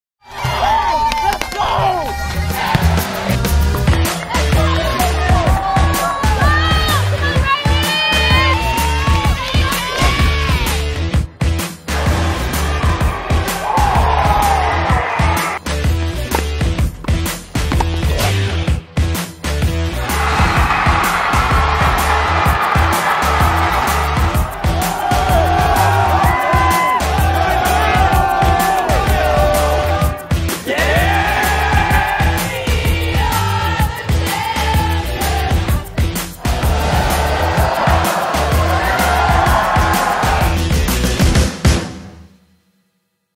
Fighting Scots Athletics
Just a peek into our Fighting Scots Athletics at Monmouth College. 20 varsity teams, NCAA Division III, one great college.